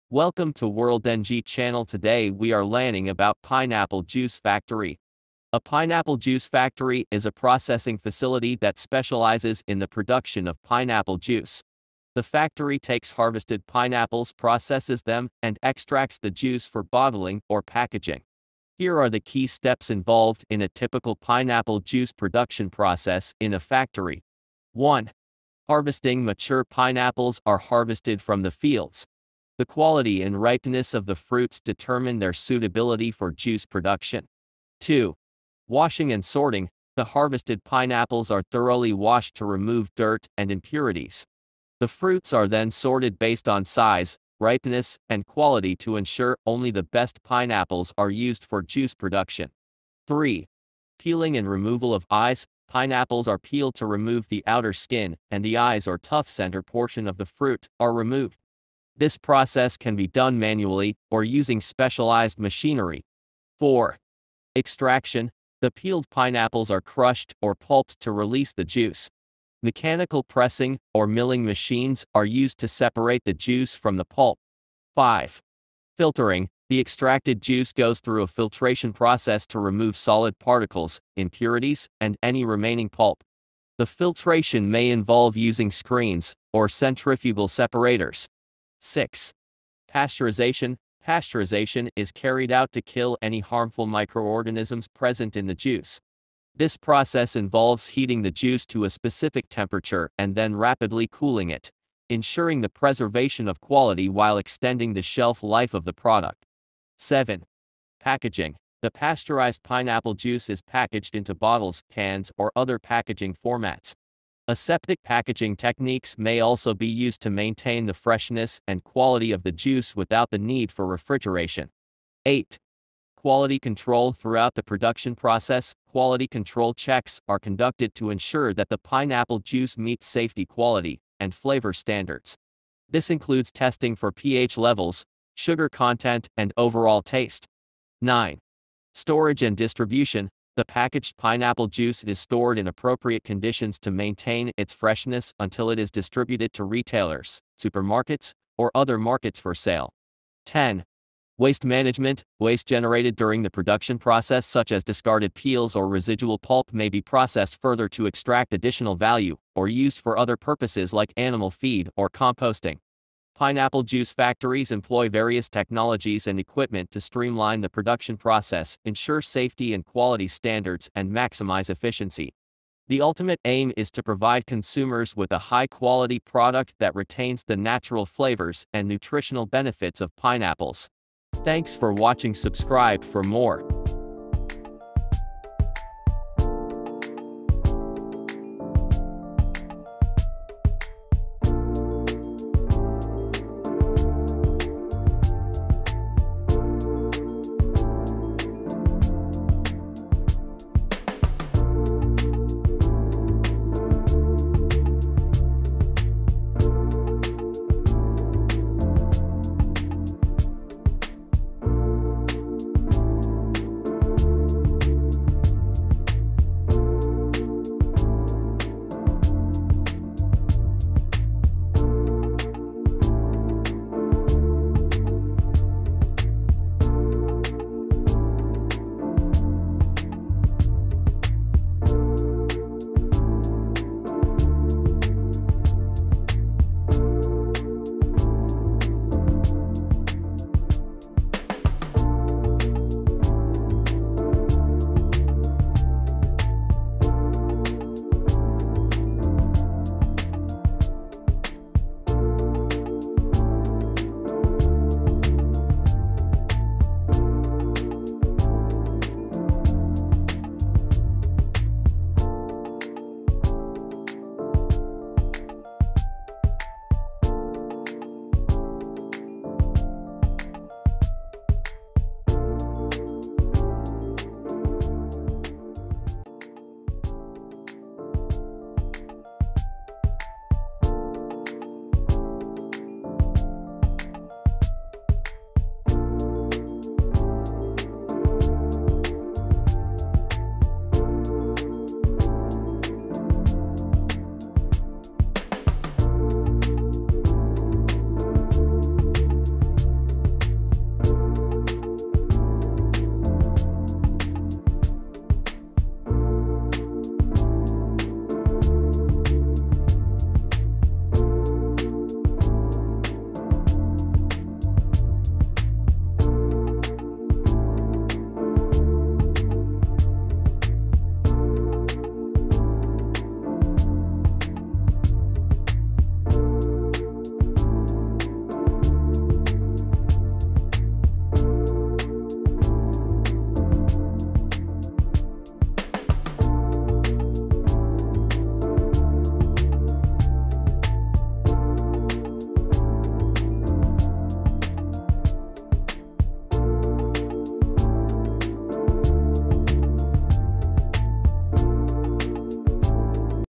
How Pineapple juice is made in factory
A pineapple juice factory is a processing facility that specializes in the production of pineapple juice.